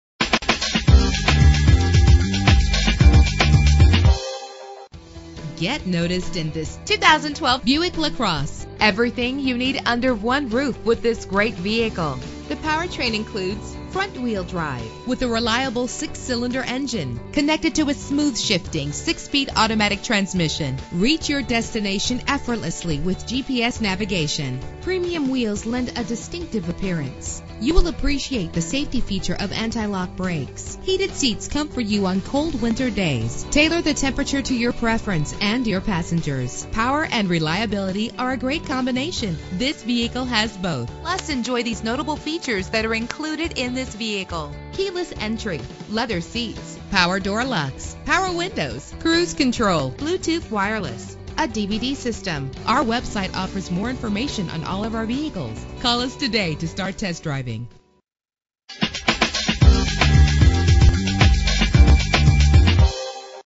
2012 Buick LaCrosse - Chittenango NY
Year: 2012
Make: Buick
Model: LaCrosse
Trim: Premium 1 Group
Engine: 3.6 liter 6 cylinder 24 valve
Transmission: 6-Speed Automatic
Color: Quicksilver Metallic
Mileage: 41056
Address:
104 West Genesee Street